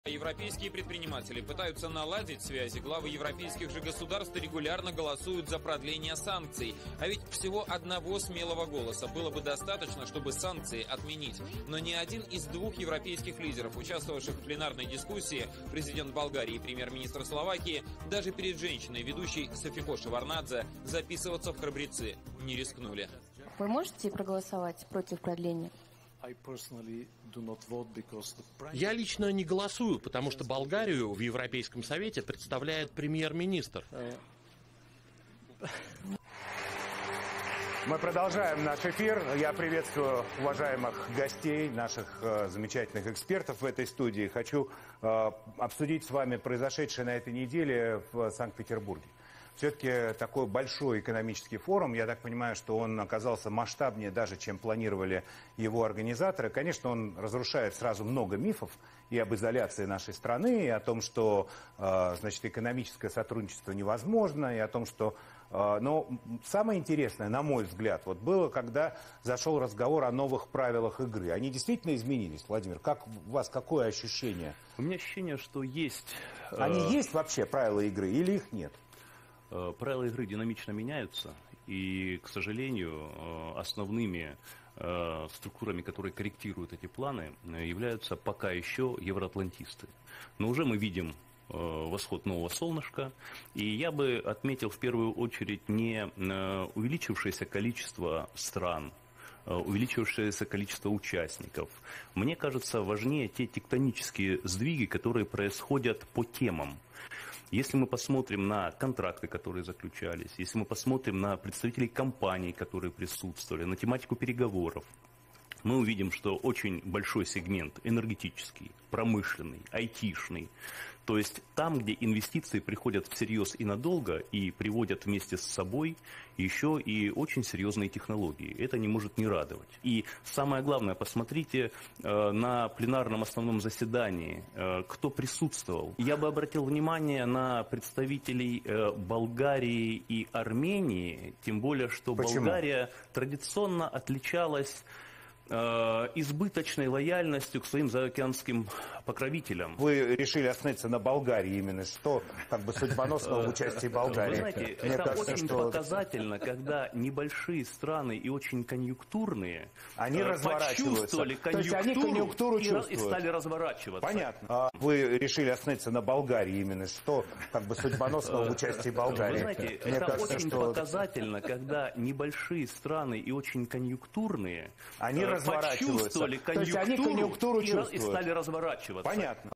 Русия продължава да се гаври с България, заради Румен Радев
За трети път в рамките на четири дни централната новинарска емисия на най-гледаната руска телевизия излъчва репортаж с подигравателно и надменно послание към българския народ, заради сервилното поведение на президента Радев, който още първият ден заяви, че изграждането на АЕЦ "Белене" било неизбежно.